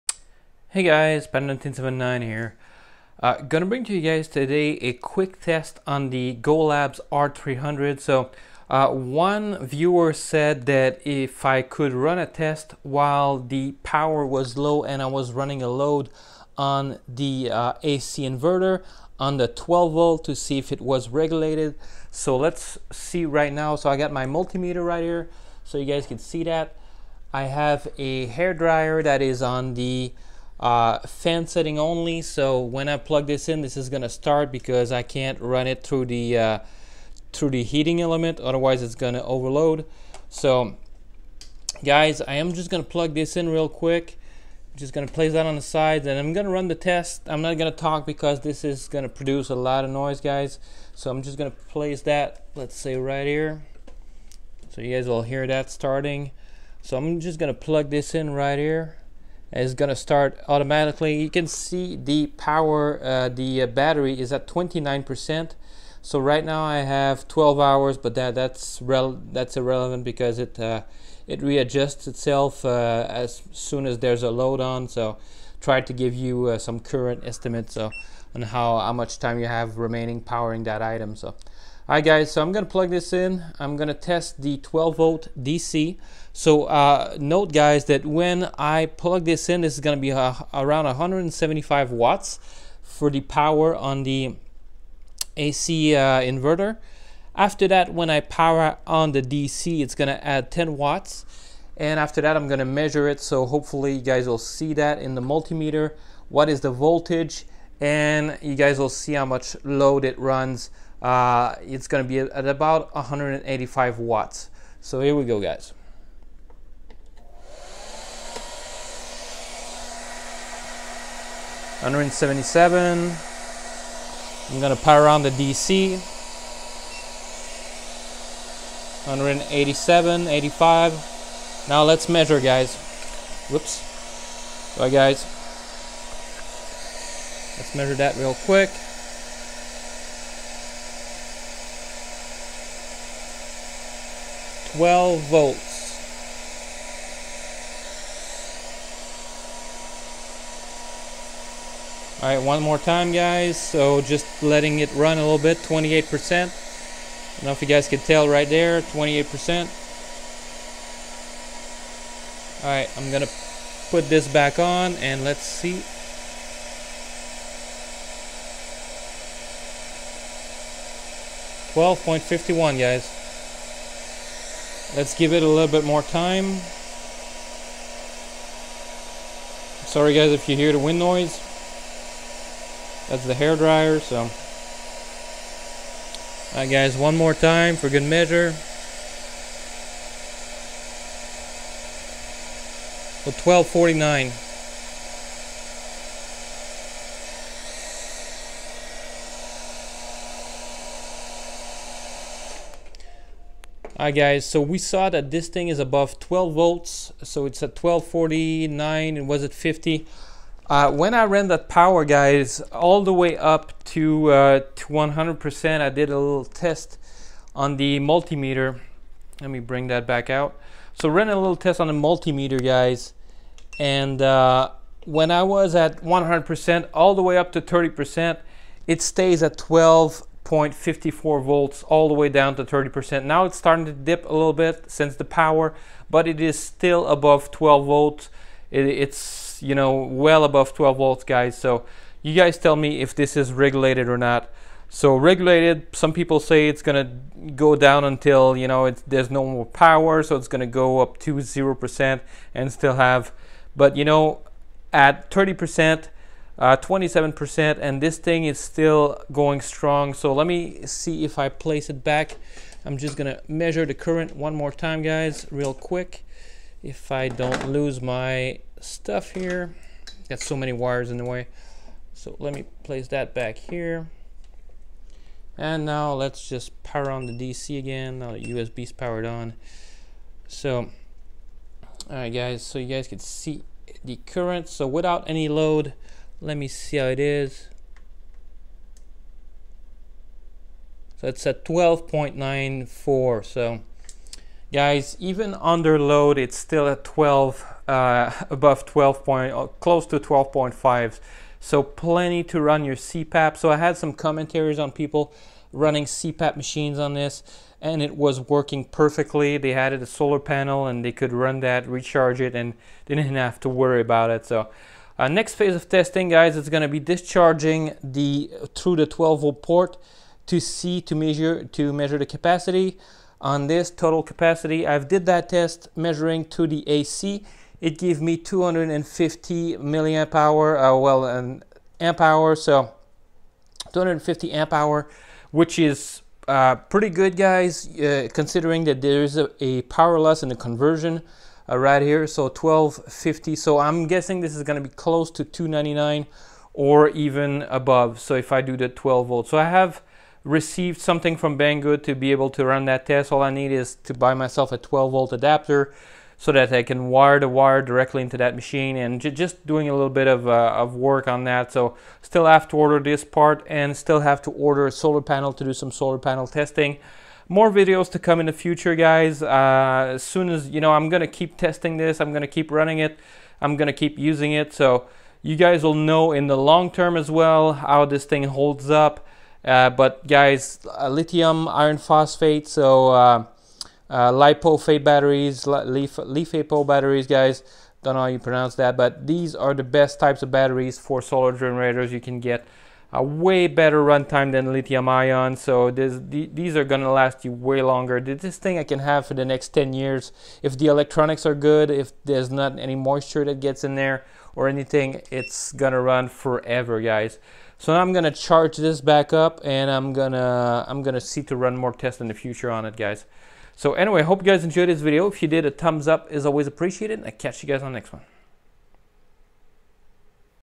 Golabs R300 12V port testing while under load and the battery is at 30%
Hey guys testing the Golabs R300 while under load you can see that it is regulated and it can run your electronic without any issues 👍